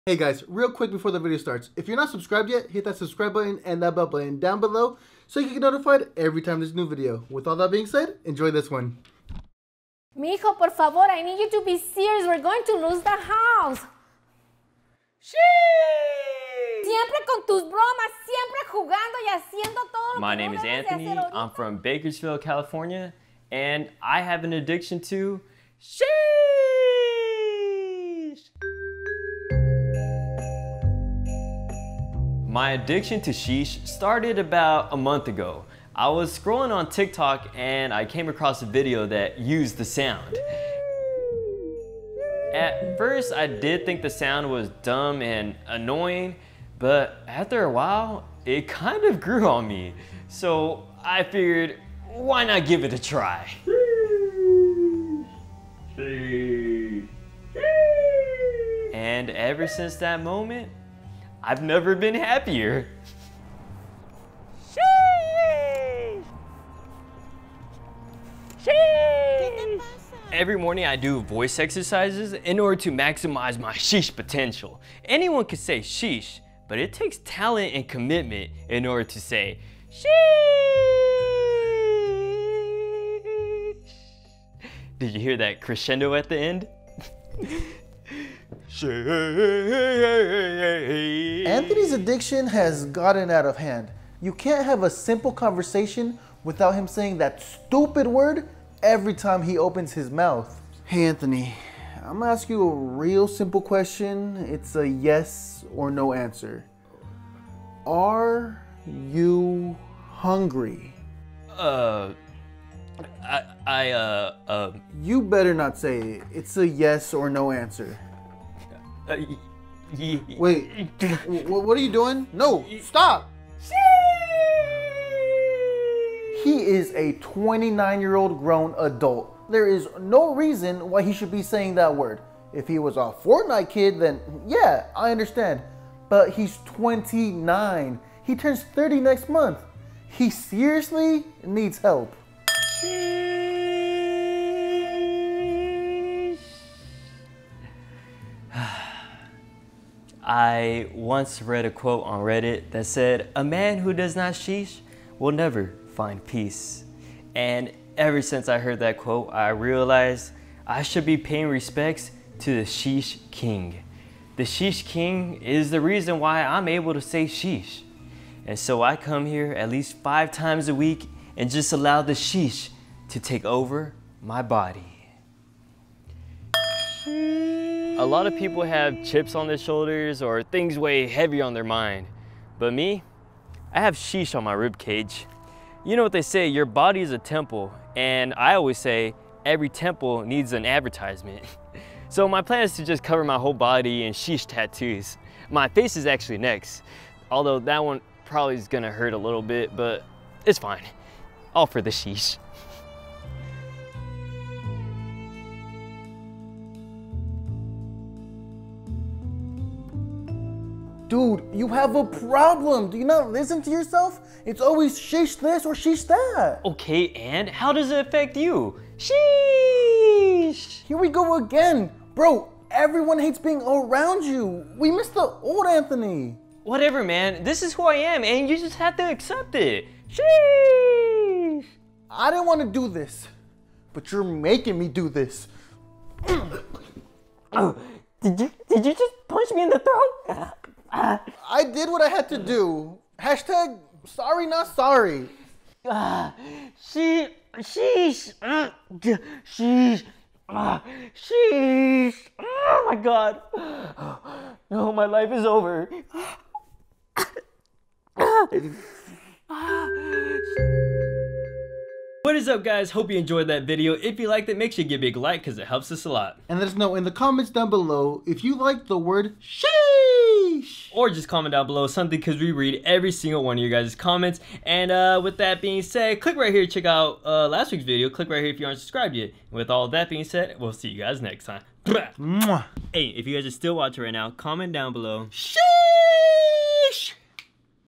Addicted to SHEESH
Anthony has a strange addiction to 'sheesh'-ing. The phrase that was popularized on TikTok has taken over his life. His addiction has gotten so bad that he can no longer have a conversation without saying "sheesh".